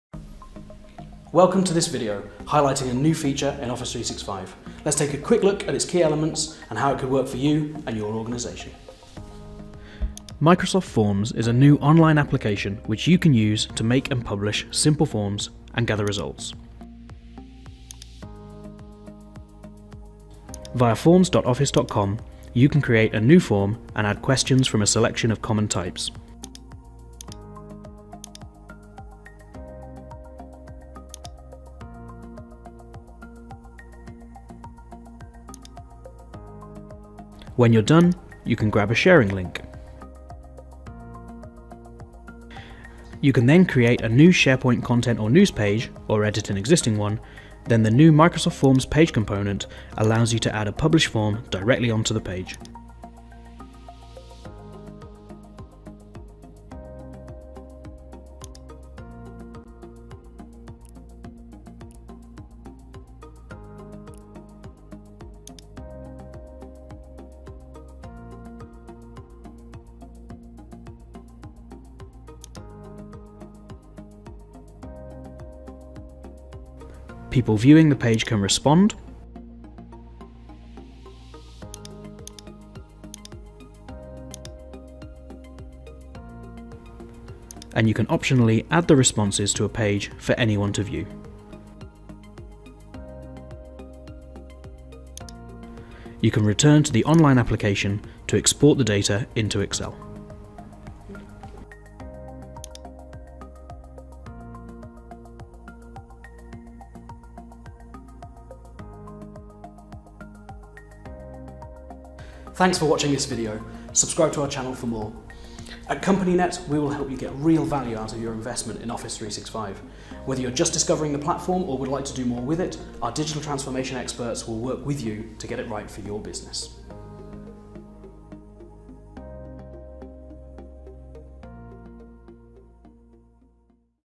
Embedding Microsoft Forms in a SharePoint page (Office 365)
Microsoft Forms is a new online application which you can use to make and publish simple forms and gather results.